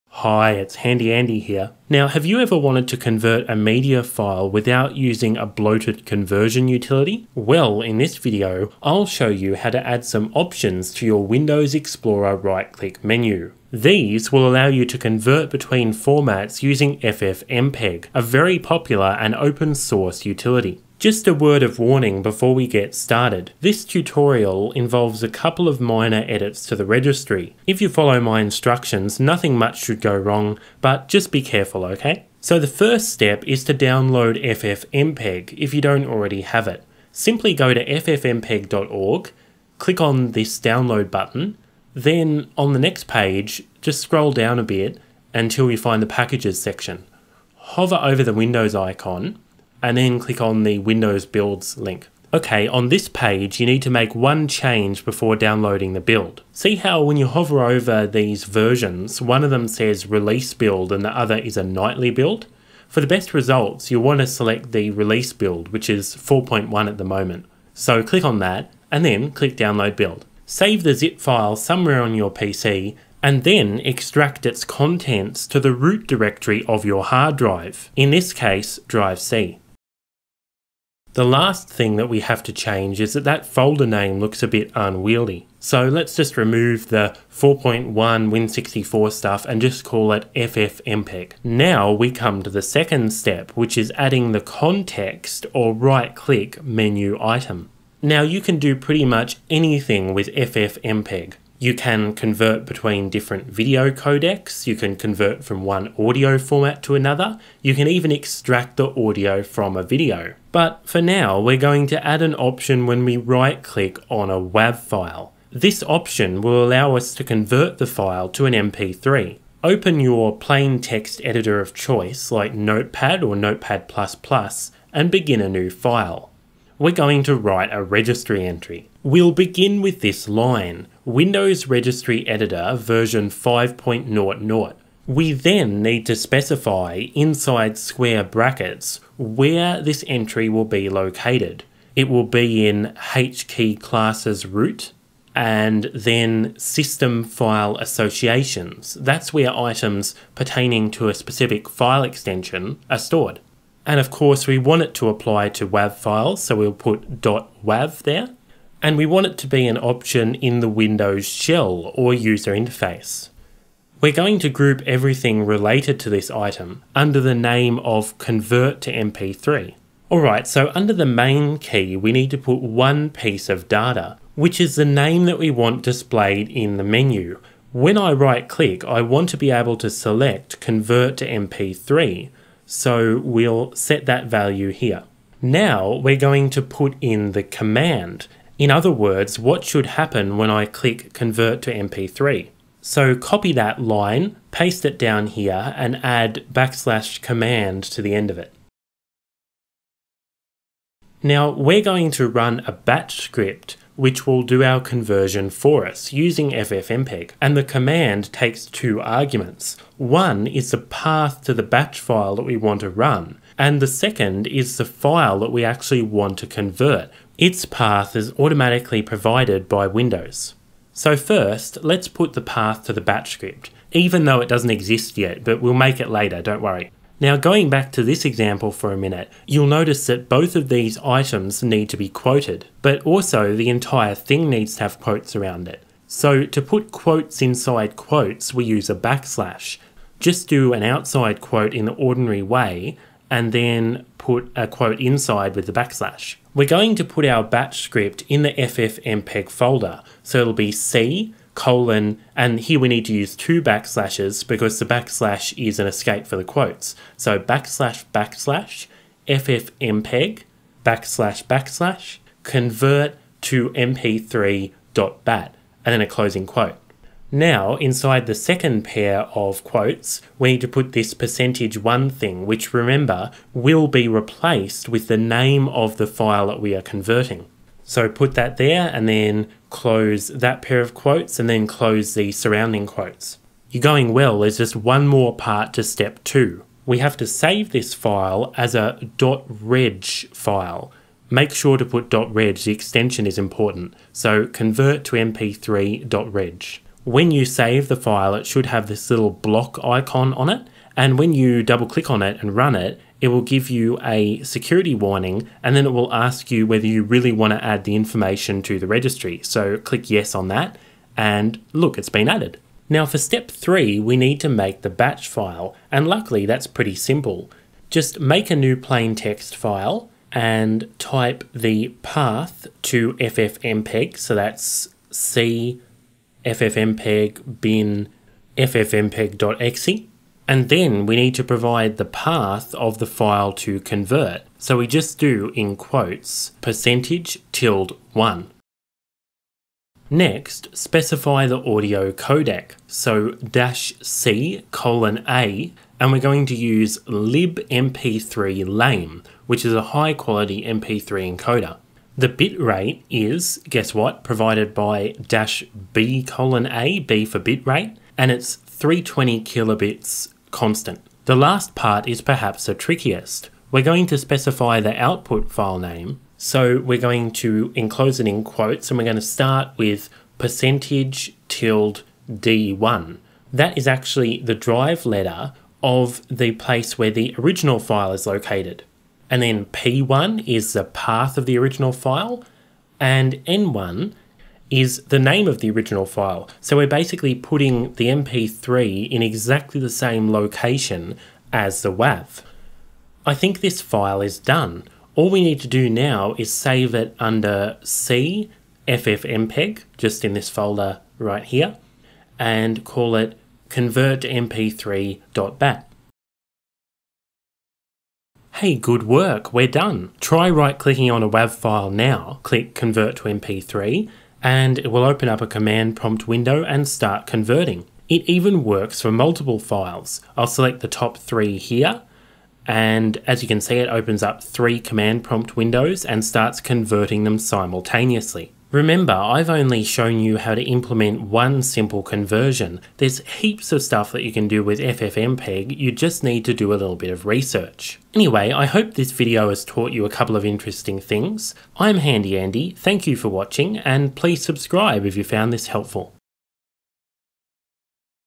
Windows 10: How to Convert Music and Videos From Right Click/Context Menu (using FFmpeg)
Have you ever wanted to quickly convert a music or video file - say, from .wav to .mp3, .mov to .mp4, or .mp4 to .mp3 - but didn't want to use a bloated third-party media converter?

In this video, I'll show you how to add options to your Windows Explorer right click / context menu. These call a batch script which converts between formats using FFmpeg - a free, open source and very popular library.

CAUTION:
This tutorial involves a few minor edits to the registry, and therefore assumes a certain level of technical skill. If you follow my instructions, nothing should go wrong, but take care.

STEP 1 (0:33)
Shows how to download, install and use FFmpeg on Windows 10. Make sure to get the Windows 64-bit release build. Extract the downloaded ZIP to the root directory of your hard drive (eg. C:)

STEP 2 (1:32)
Illustrates how to write a registry entry to add a new item to your context menu (eg. "Convert To MP3"). When this item is used, it will call a batch script which converts your media using FFmpeg.

STEP 3 (5:15)
Tells you how to write the batch script mentioned above. It is relatively simple if you're doing a straightforward conversion - in this case, from *.wav to *.mp3.

Remember that this tutorial just scratches the surface of what FFmpeg can do.

...and...